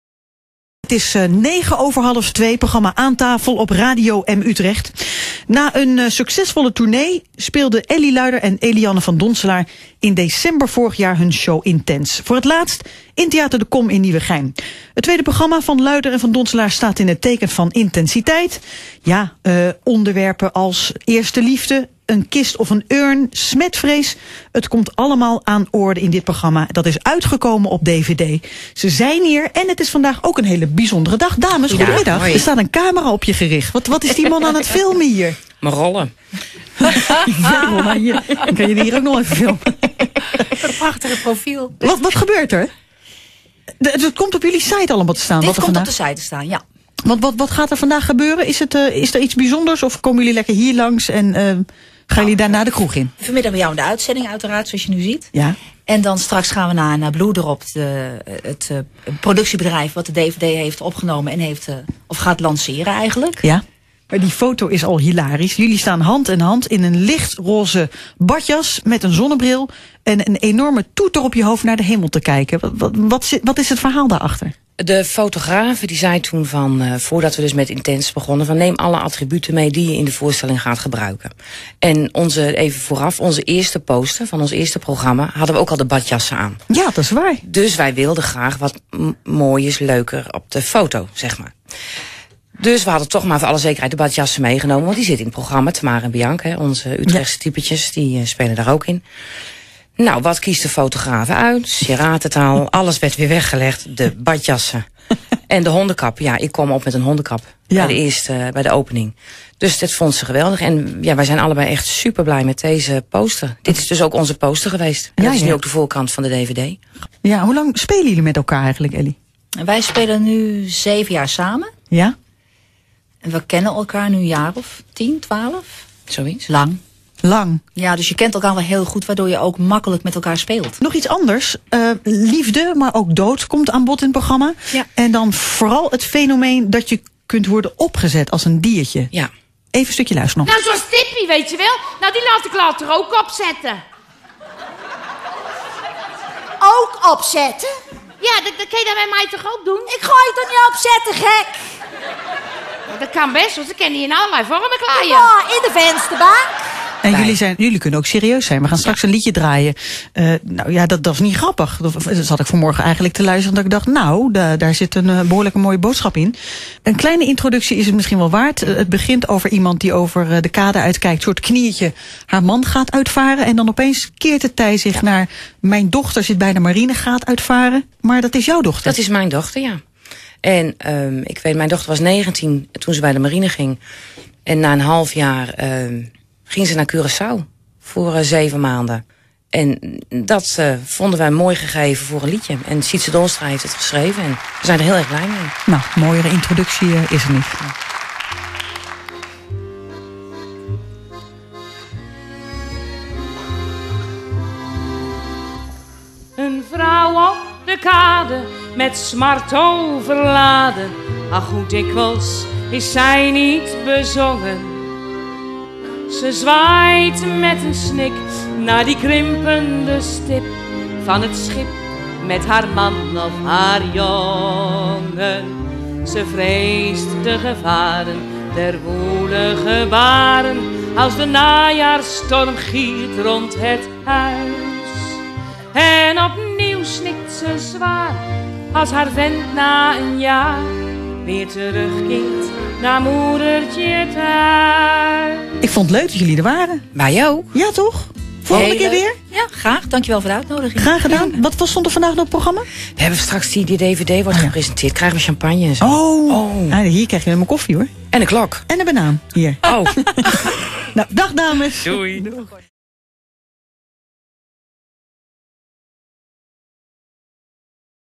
Luider en van Donselaar bij Radio M Utrecht
Utrechts cabaretduo Luider en van Donselaar op bezoek bij Radio M Utrecht. Voor meer info of of